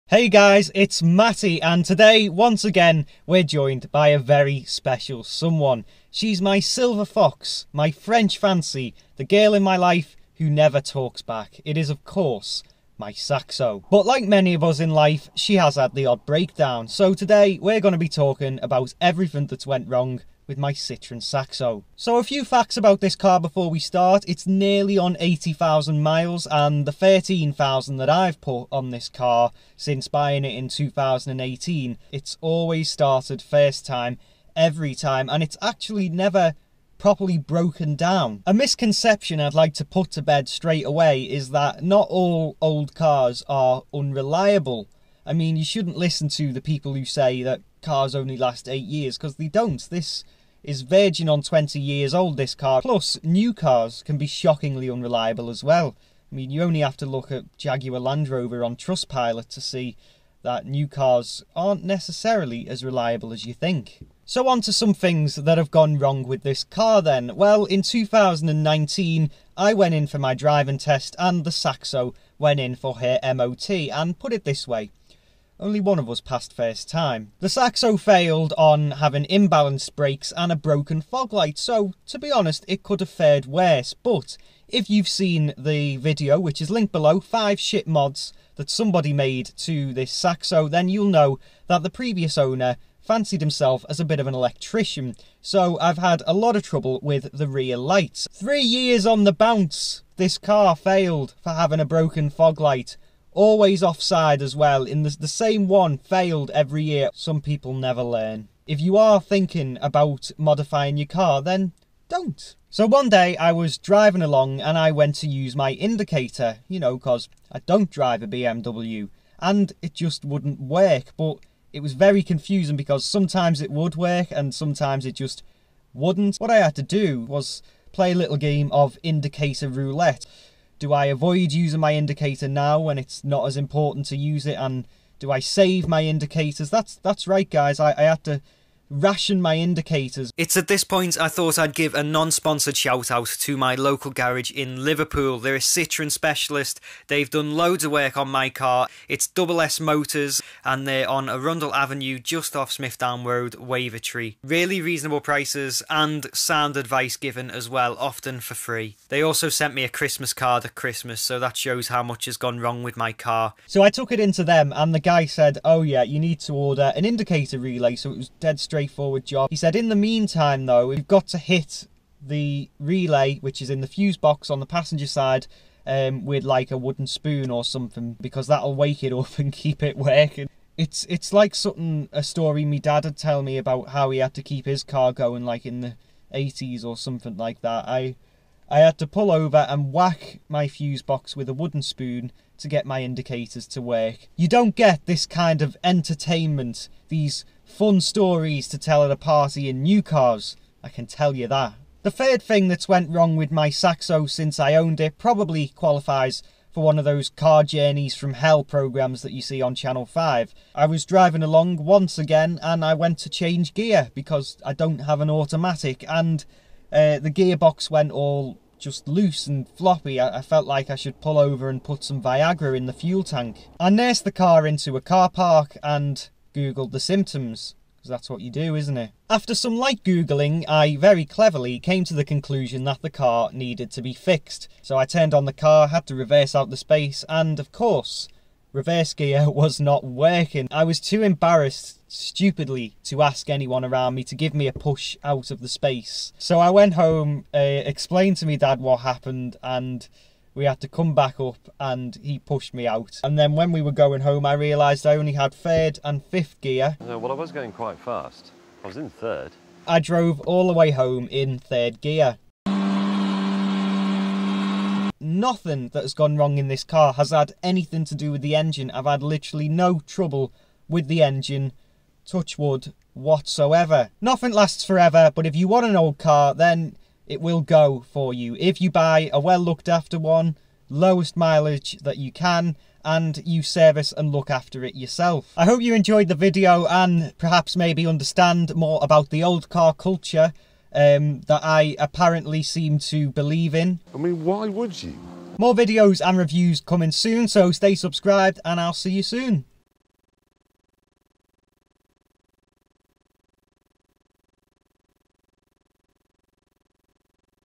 Everything That’s Gone Wrong On My Citroen Saxo - Matty’s Cars
Some story’s of my Saxo’s problems.

Drivetribe:  Matty Manning

Want me to review your car? Message me or send an email.